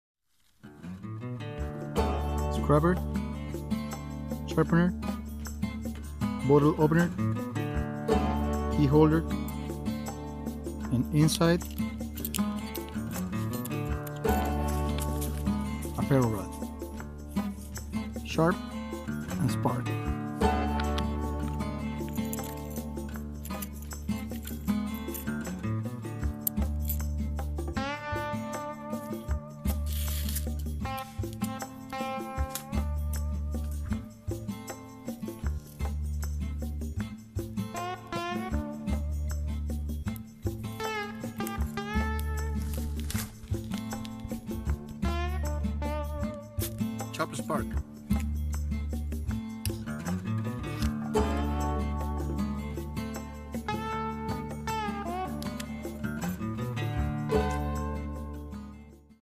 Sharp-N-Spark nice outdoor tool
Thanks to Brad Buckner for this nice tool you gave me when we met at @blade_show 2018. "SHARP-N-Spark is a very useful tool for outdoors. Get yours at @sharpensbest